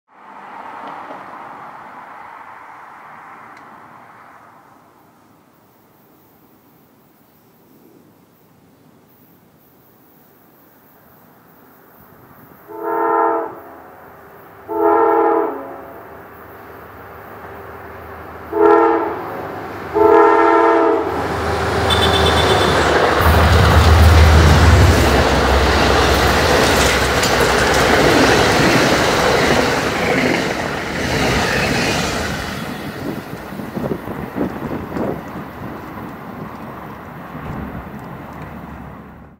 Amtrak Phase III P32AC-DM 704 Leads Amtrak [48] LSL⁴ᴷ
Amtrak's Southbound Lake Hore Limited cruises by New Hamburg with P32AC-DM #704 leading. P32 #704 is the first of Amtrak's P32s to receive the new "Amtrak America" Phase III paint scheme. In addition to the scheme the locomotive also features an "Empire Service" logo on each side. All 18 P32AC-DMs will slowly be repainted one by one. (Taken 1/13/16)